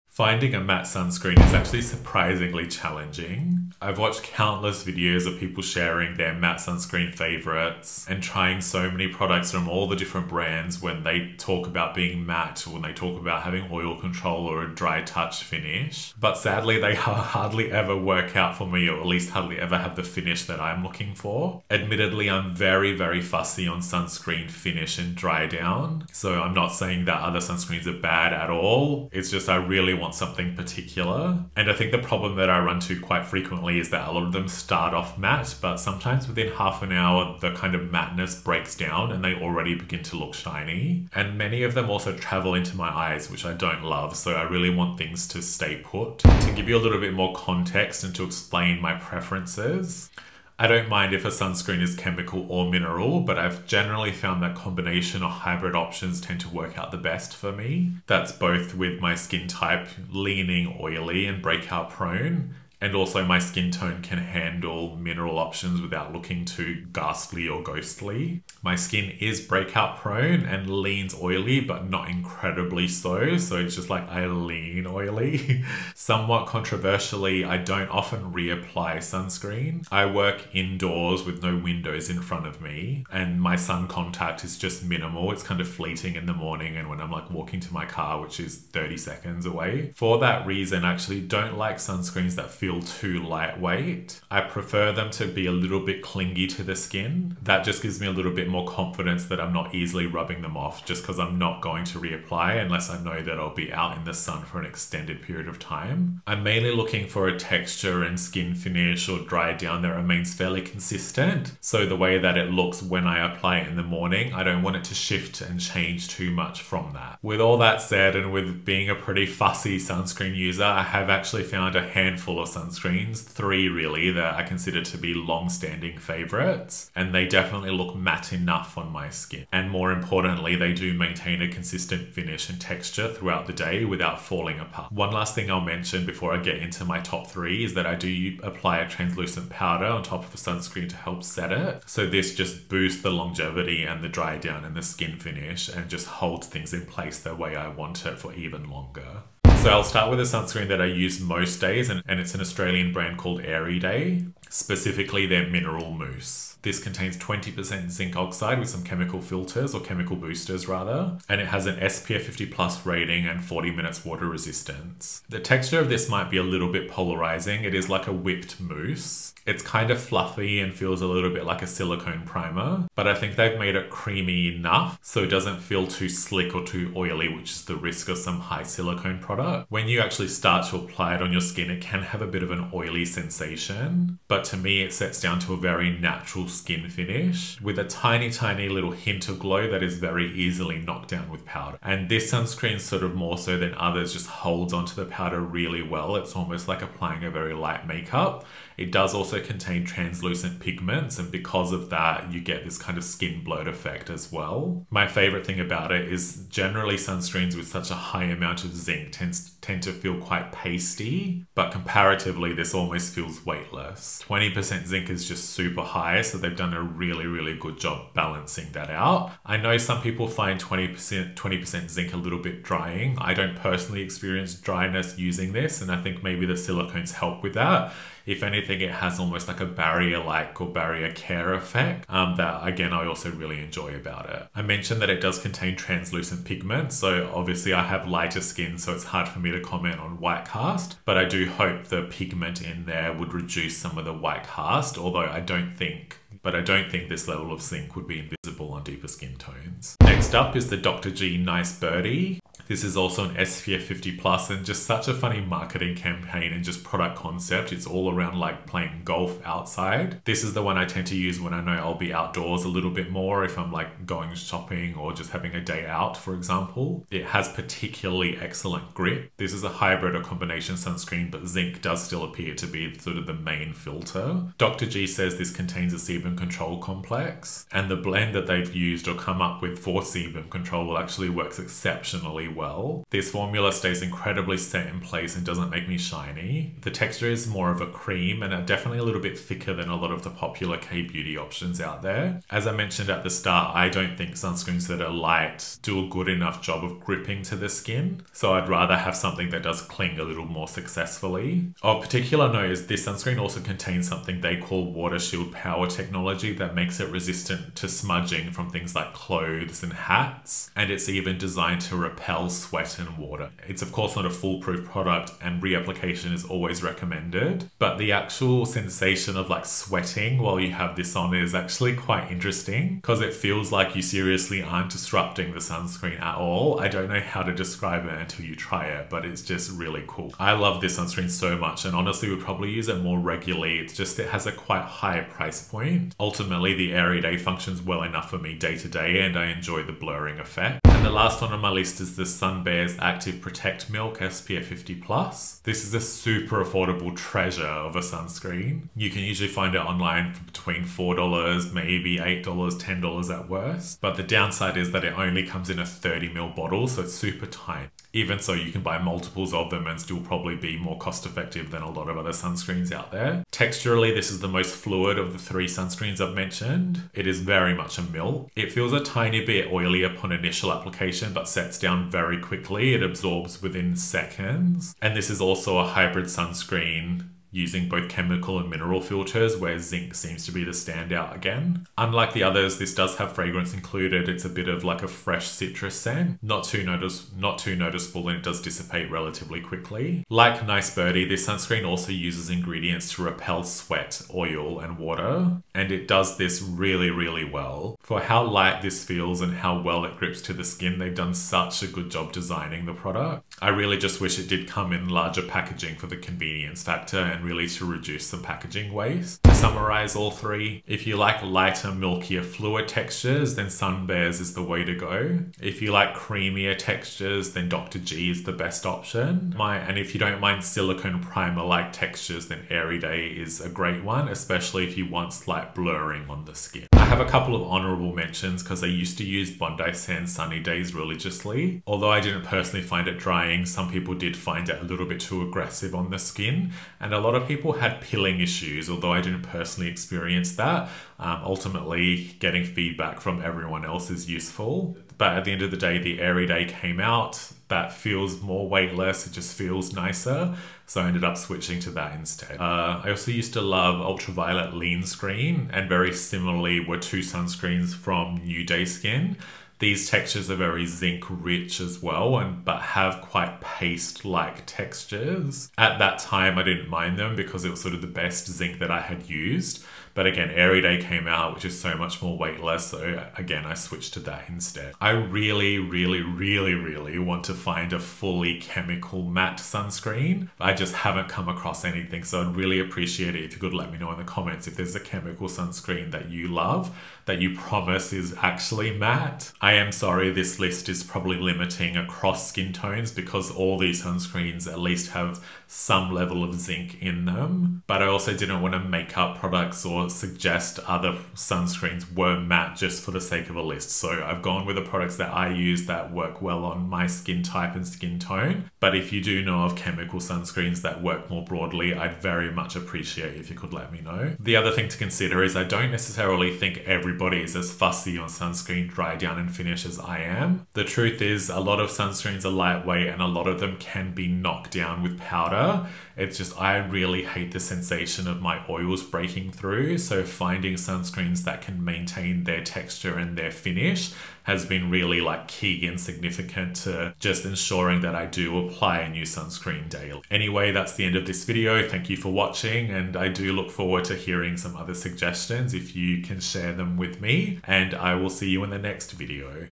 3 Actually Matte Sunscreens
Matte SPF products are hard to find, this is my list of three sunscreens that actually look matte enough on my skin.

Products include Airyday Mineral Mousse, Dr. G Nice Birdie, and OMI Sunbears Active Protect. All of them are SPF 50+.

Please do let me know your matte favourites in the comments.

Timestamps:
00:00 Introduction
00:47 My SPF preferences
02:30 Airyday
04:10 Dr. G
05:41 OMI
06:49 Quick Summary
07:07 Honourable Mentions
08:36 Outro